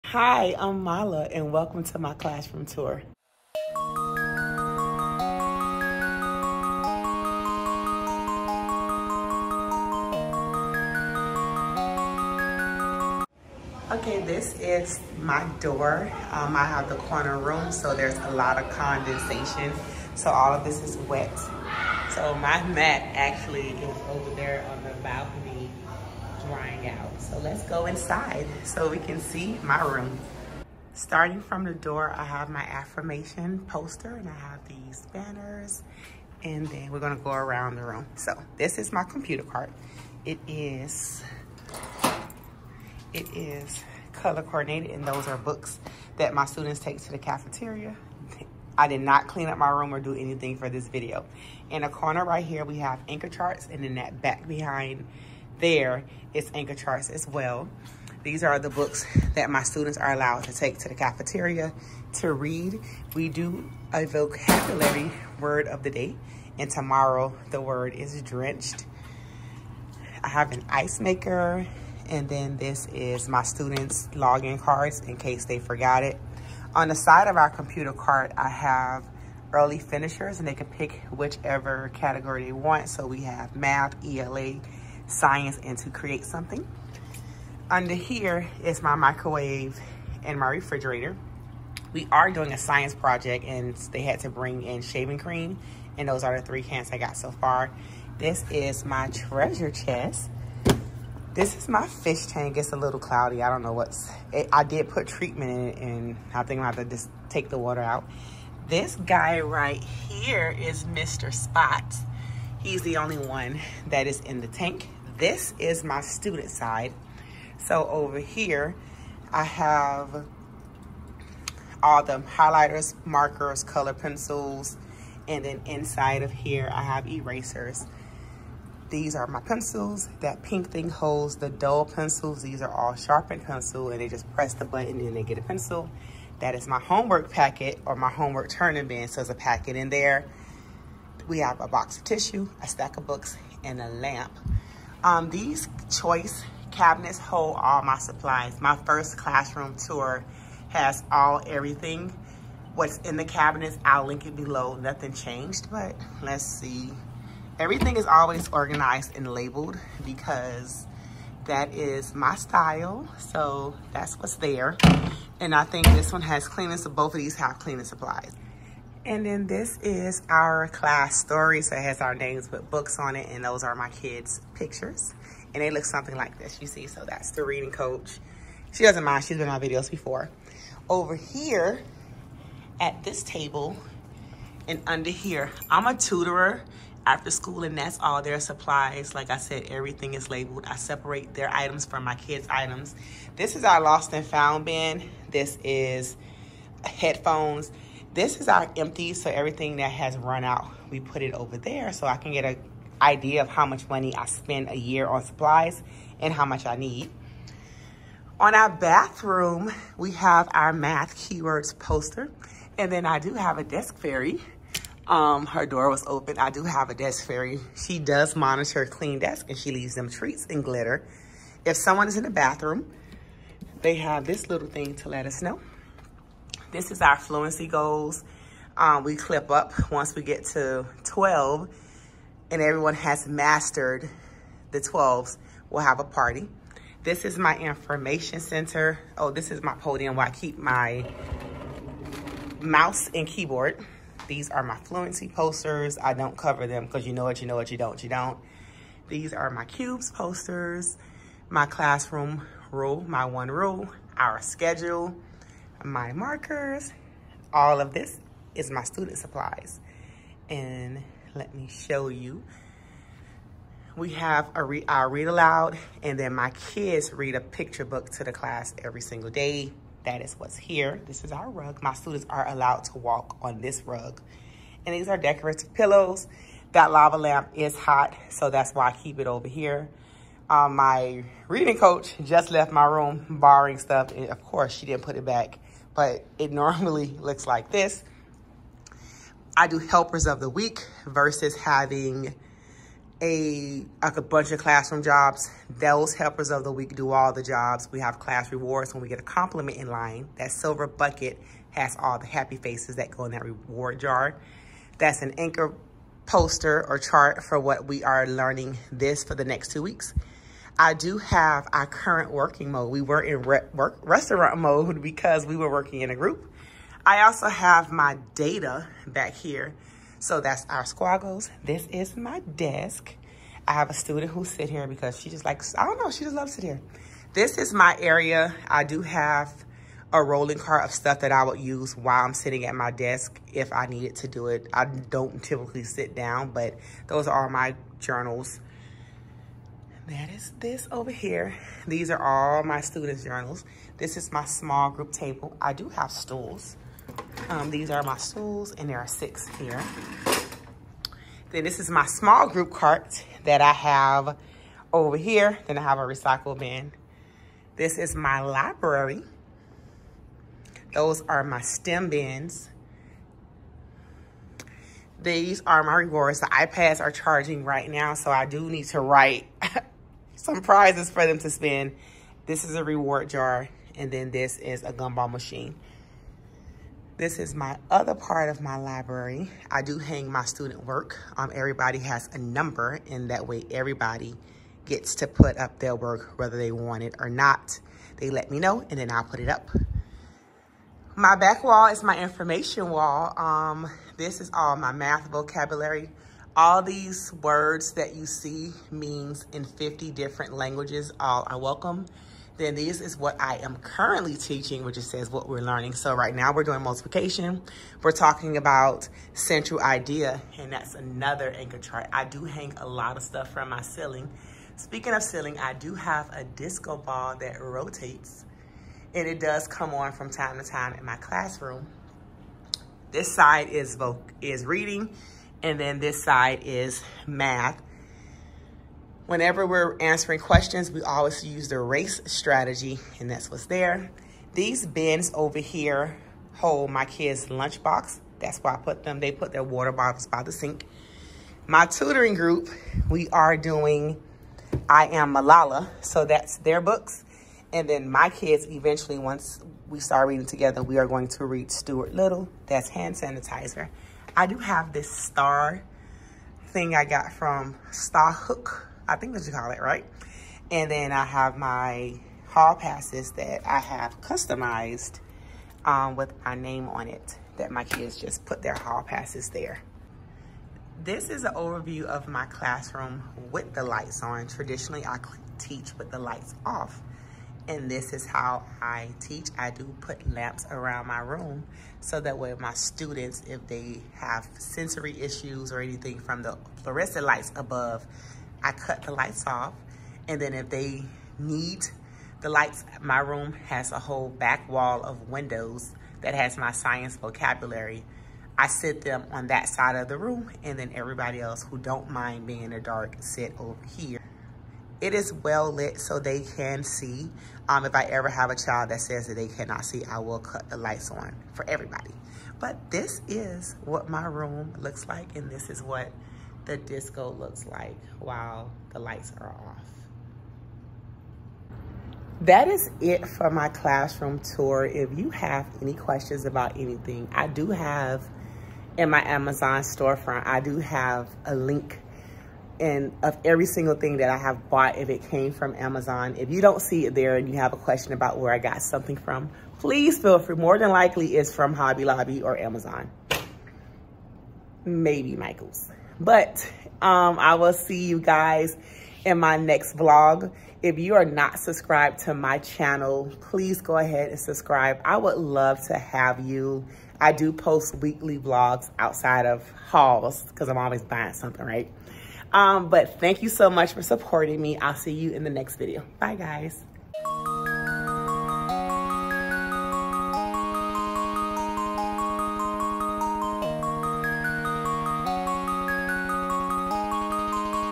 3rd Grade Classroom Tour | 2024-2025 School Year
Hello internet friends, teachers, teacher to be, students and people who just like classroom vlogs and decor. Today I am sharing a week in my 3rd grade classroom for the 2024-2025 school year.

I promise you’re gonna love it here!!!

 ☆ CLASSROOM WISHLIST:

☆ AMAZON STOREFRONT: